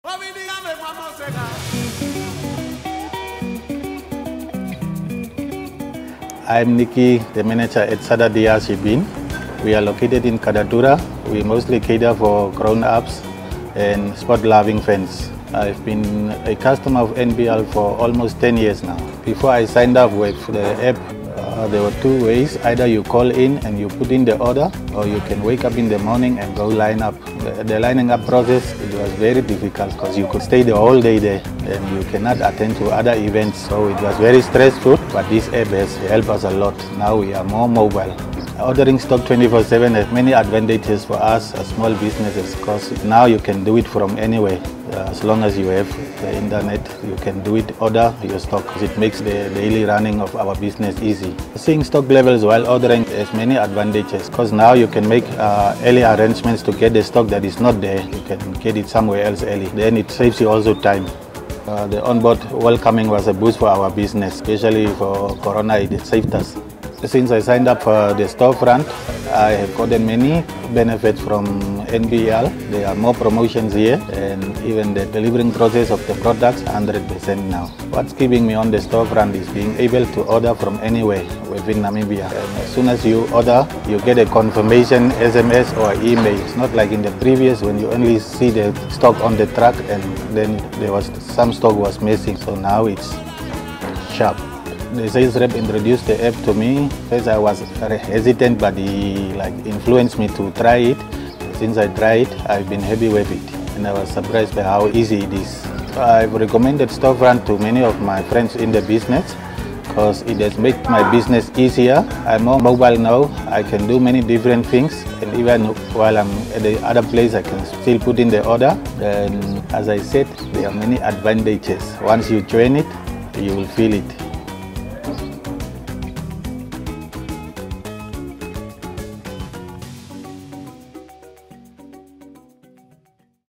NBLStorefront, Impact on Small Businesses in Katutura, Windhoek
NBLStorefront is the Namibia Breweries Limited (NBL) online shopping platform that is available to all registered NBL Customers.

NBL Customers now have the ability to do a full product search, add items to their cart, and submit orders at their fingertips. This means that Customers no longer have to wait in queues to place orders or track fulfilment. What’s more, Customers can now check the health of their financial accounts with NBL in real time*.

The NBL DigitalTransformation team spoke to Nicky Skrywer, owner of Sada-Tia Shebeen, about the impact of NBLStorefront on his entrepreneurial journey: “There are many advantages to joining NBLStorefront; once you're a part of it, you will feel it."